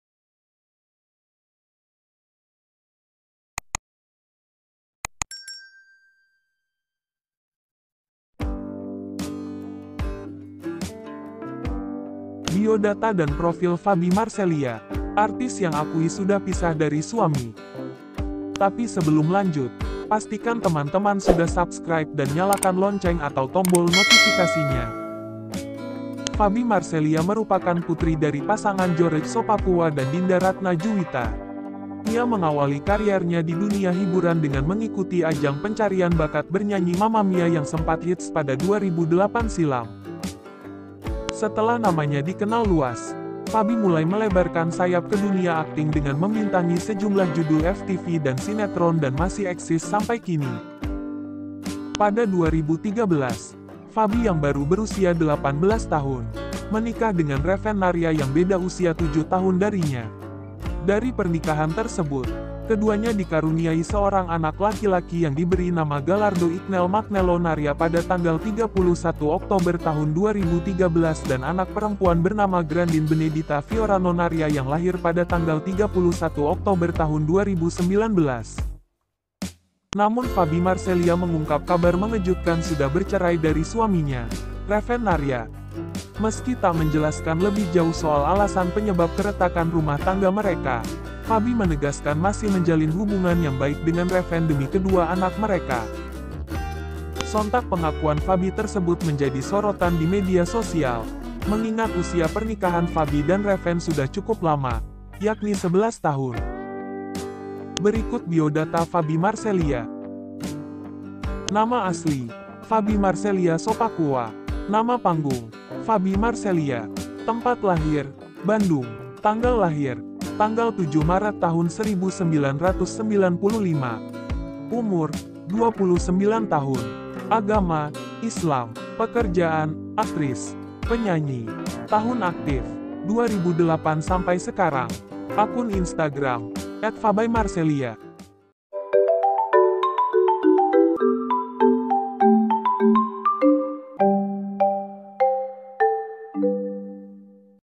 BIODATA DAN PROFIL FABY MARCELIA • ARTIS YANG AKUI SUDAH PISAH DARI SUAMI
terimakasih udah menonton,
maaf jika ada kesalahan pengetikan, penyebutan dan lain-lain.

Source:
• Instagram Faby Marcelia